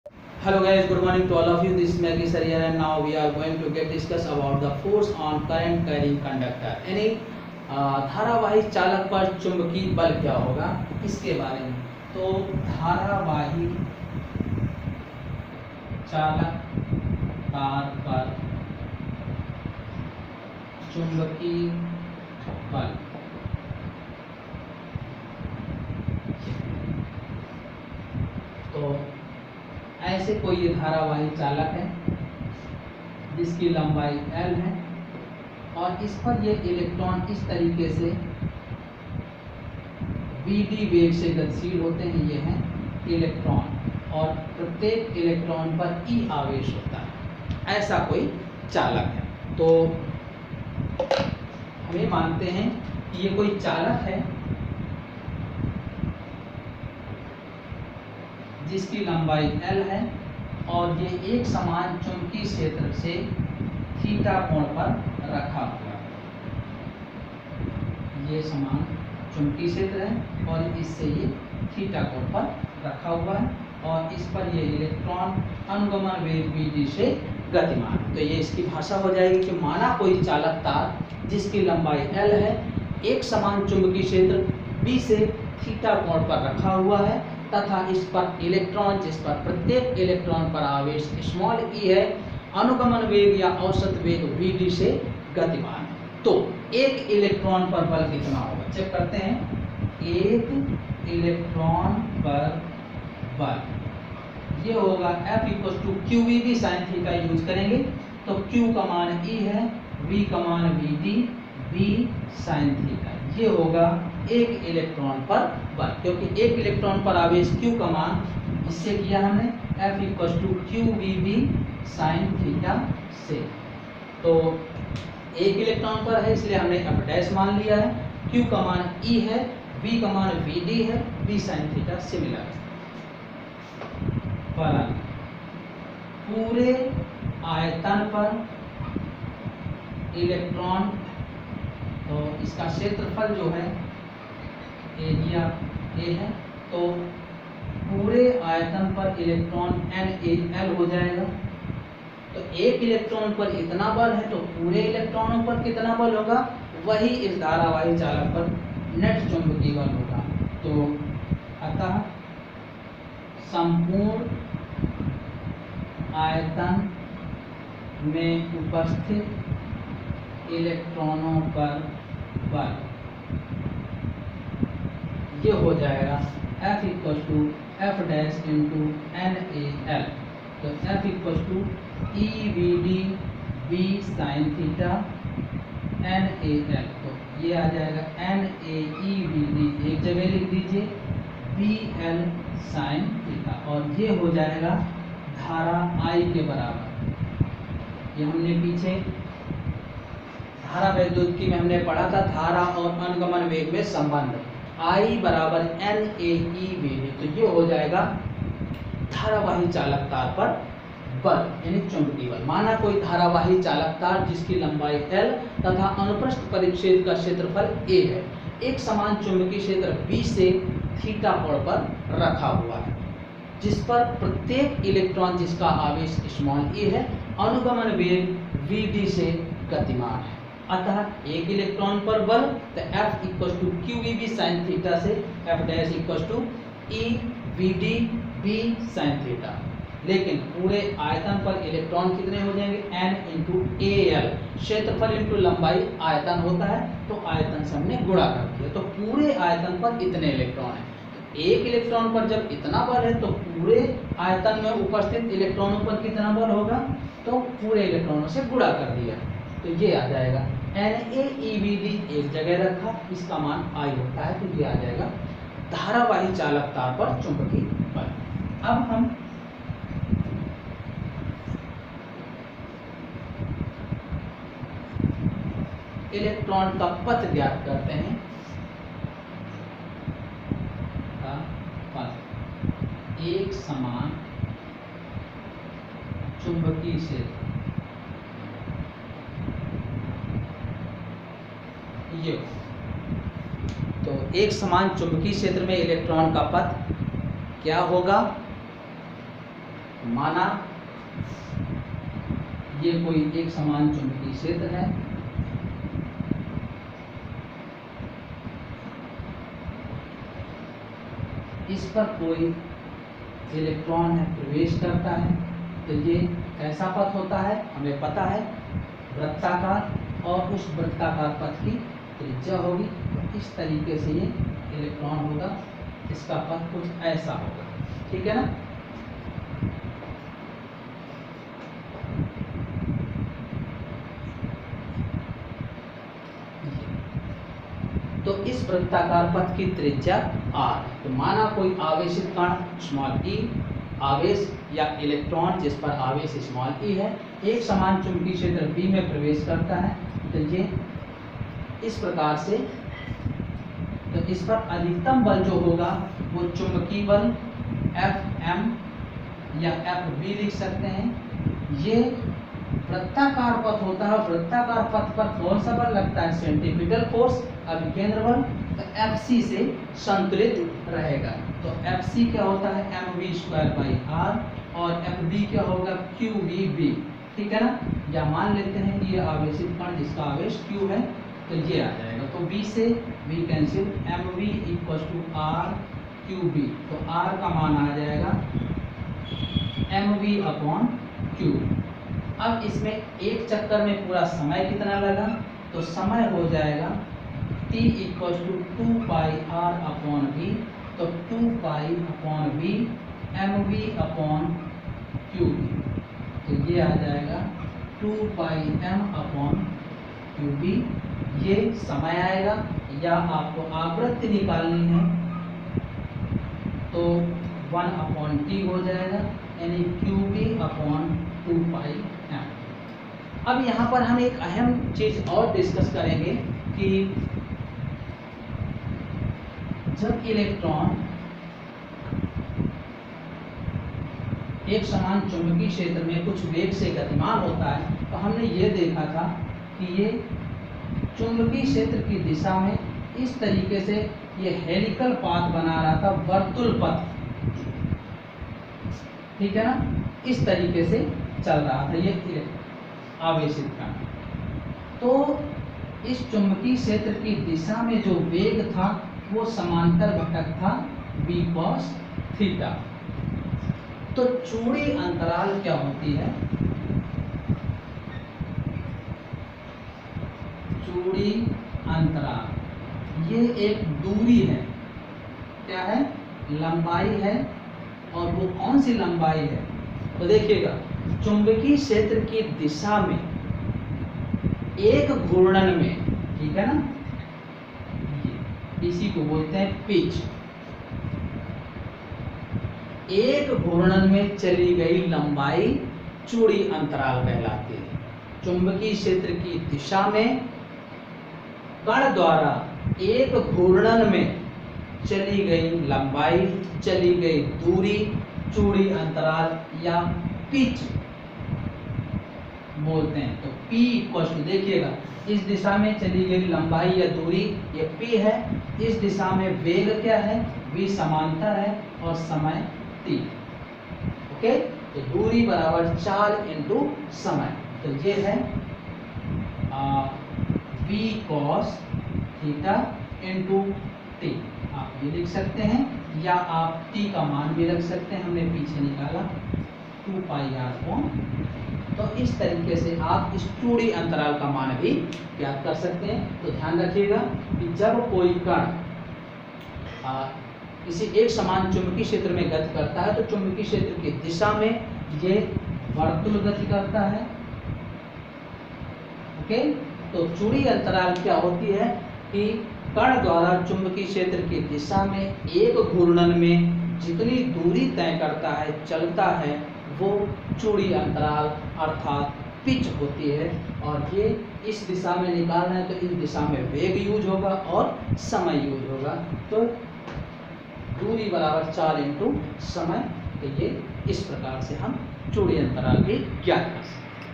Physics by Maggi Sir (class XII)hindi medium
Topic..गतिमान आवेश और चुम्बकीय क्षेत्र (भाग 03)
धारावाही चालक पर चुम्बकीय बल, चुम्बकीय क्षेत्र मे इलेक्ट्रॉन का पथ,त्रिज्या, समय ,चूडी अन्तराल।